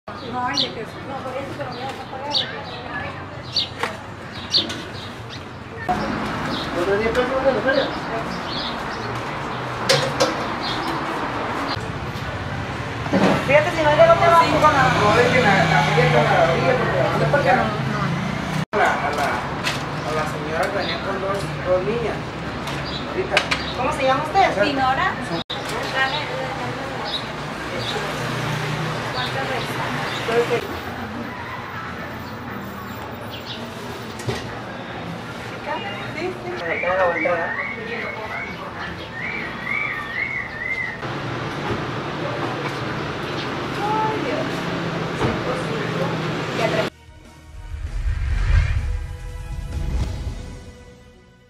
Doña Silvia da el sazón al Día de la candelaria con sus tamales
Guasave, Sin. Desde hace casi 30 años los tamales hechos por Doña Silvia como es popularmente conocida, están presentes en la mesa de miles de comensales y al tratarse  del tradicional Día de la Candelaria con mayor razón.